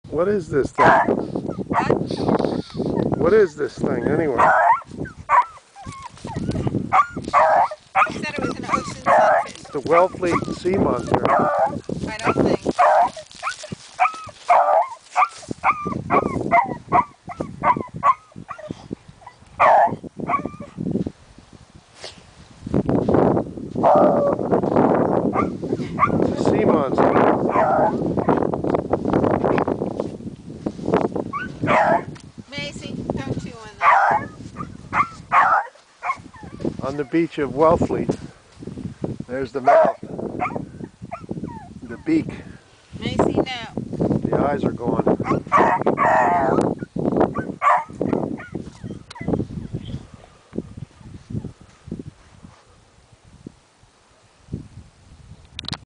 Wellfleet Sea Monster?
This 7' long 600lb (est) monster washed up on the beach at Chipmans Cove, Wellfleet, MA in Nov 2009.